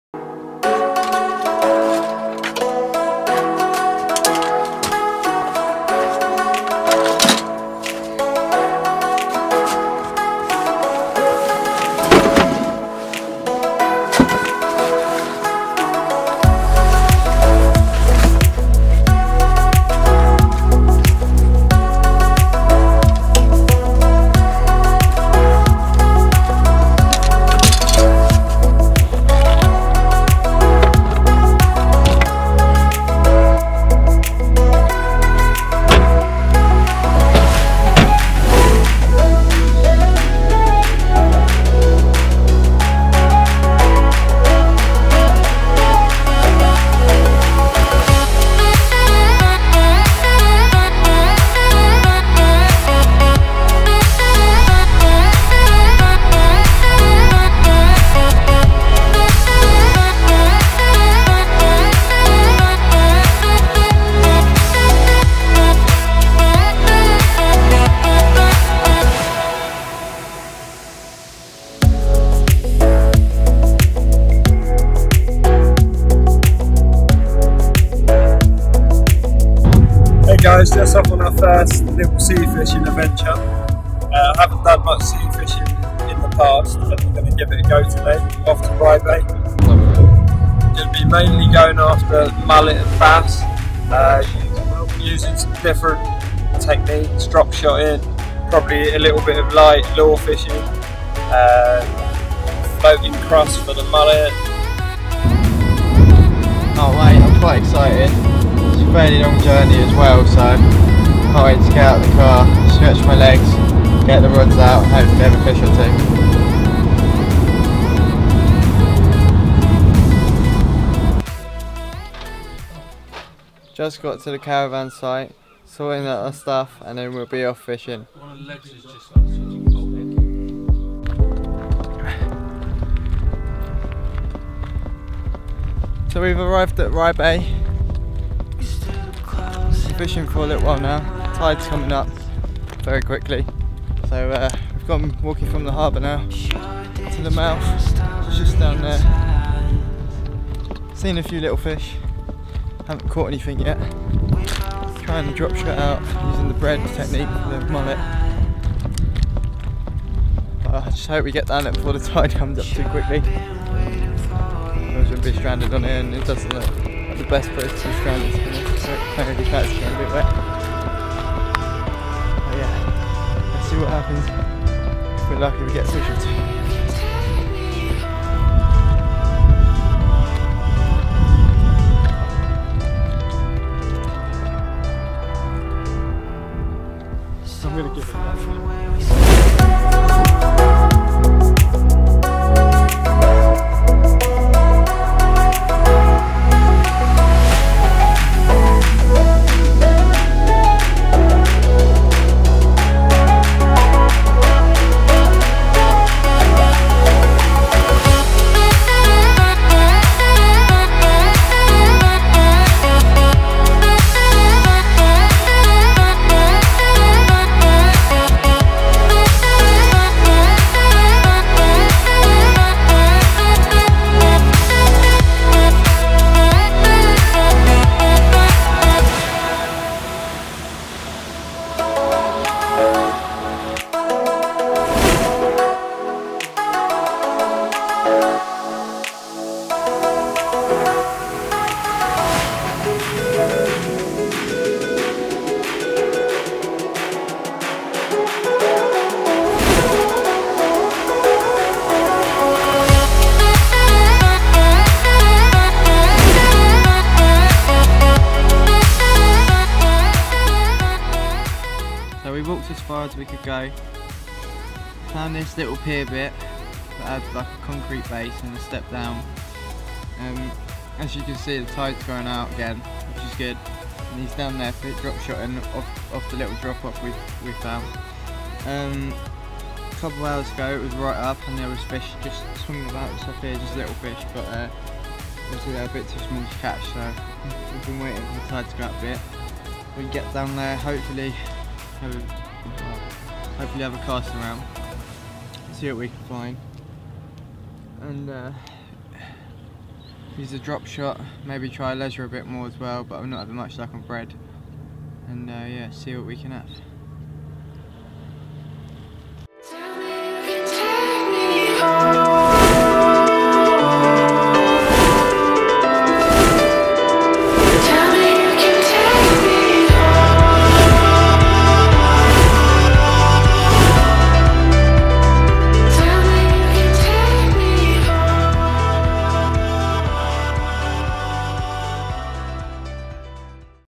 Sea Fishing - Rye Bay
We decided to take a weekend away from fishing freshwater and organised a little trip to Rye Bay.